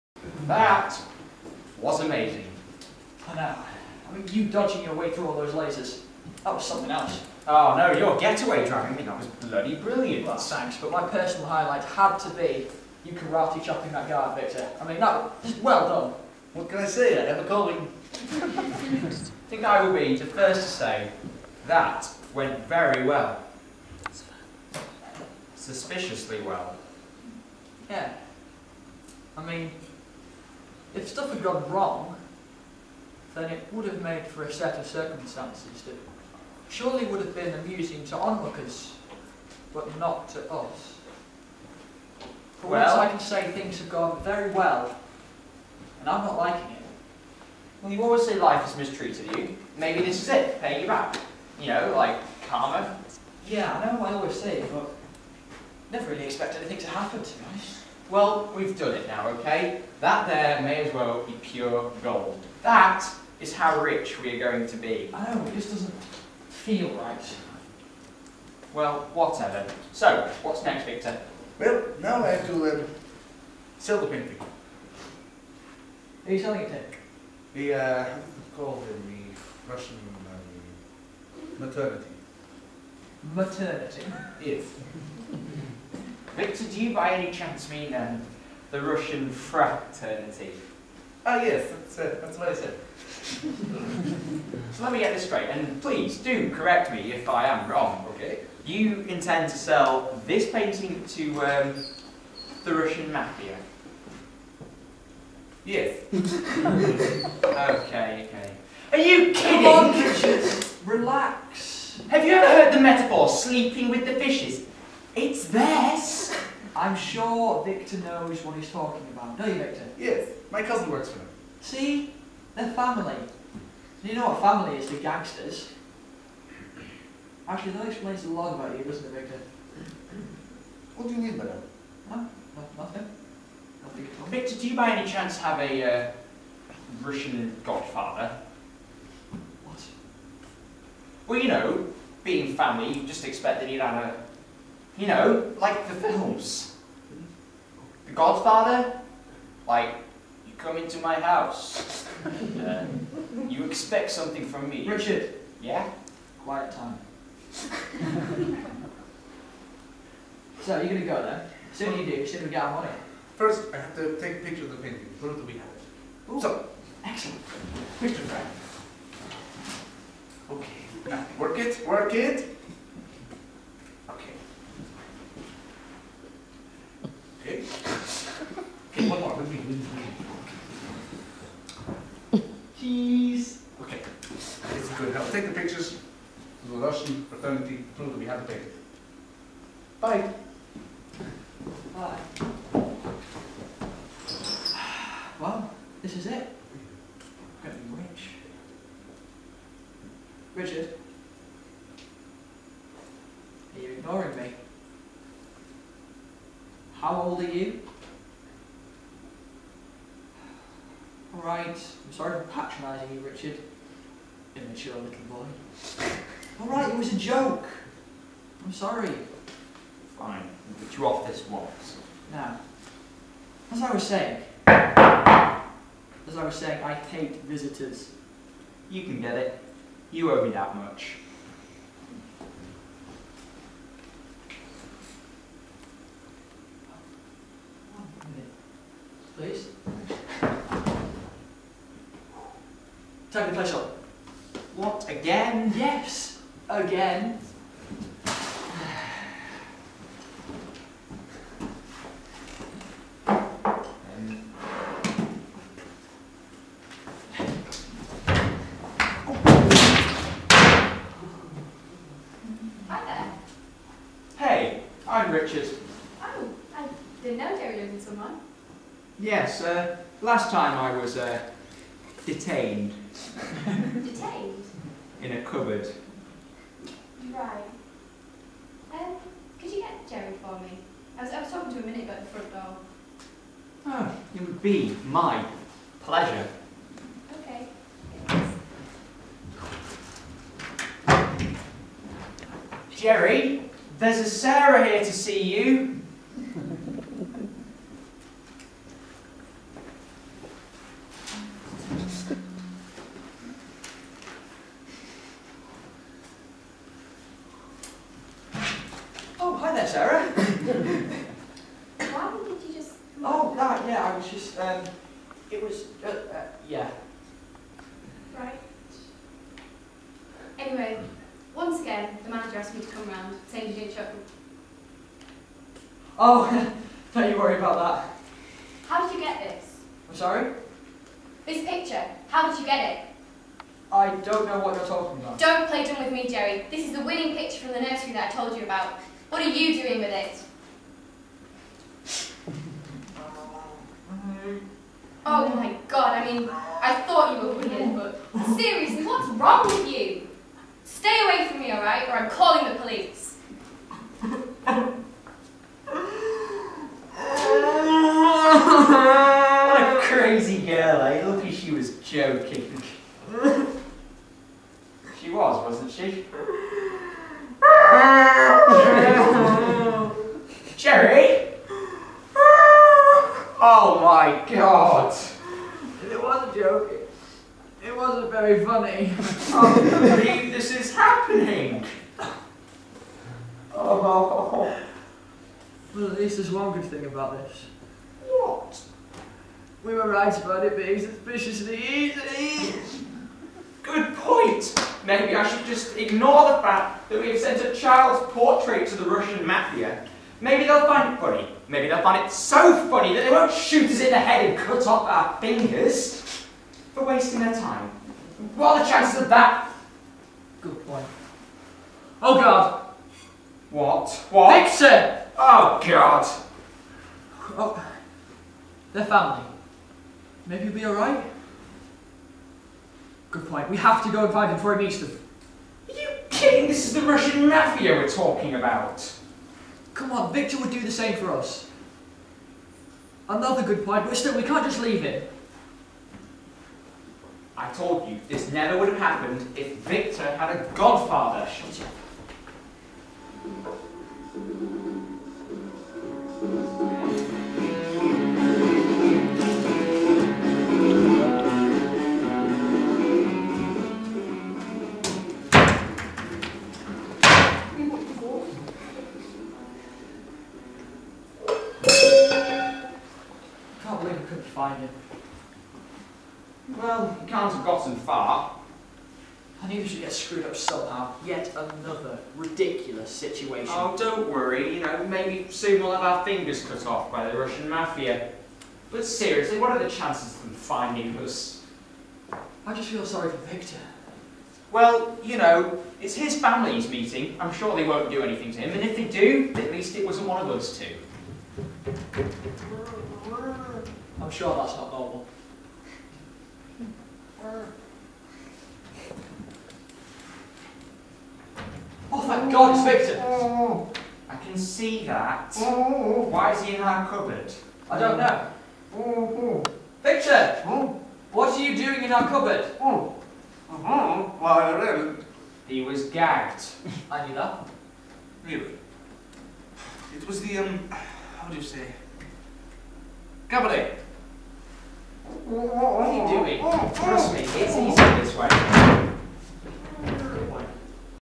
The Perfect Copy (Act II, part 1)
From the play "The Perfect Copy", written by Sean Martin and performed by:

Richard - Kenny Edwards
Jerry - Adam Whitehead
Viktor - Marcus Rudd
Sarah - Emily Edwards
Gustaph - Jaime Monfort Miralles
Mikhail - Huw Roberts

Thank you, M.H, for filming it :)